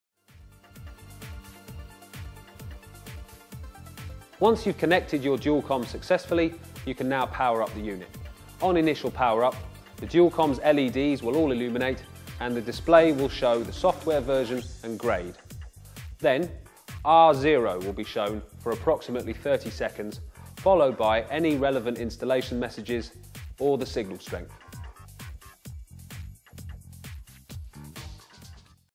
CSL DualCom Gradeshift training video - Initial Power Up
One of the videos made available by CSL DualCom to help installers looking for information on its DualCom Gradeshift product. Find the full range of training videos